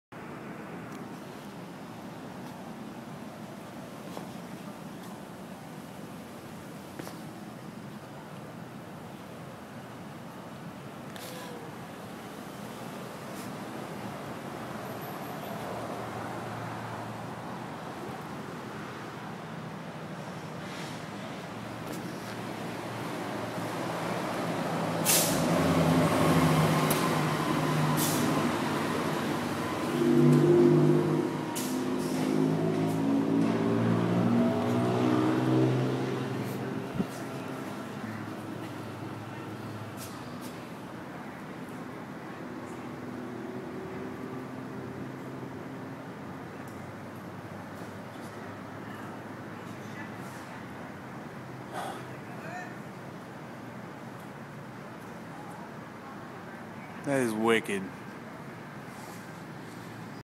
Cory Duncan tries the puzzle in LA
That crazy light up game on the ground in front of an apartment building in LA. If you watch the whole thing you will notice the front of the building lit up just like the puzzle!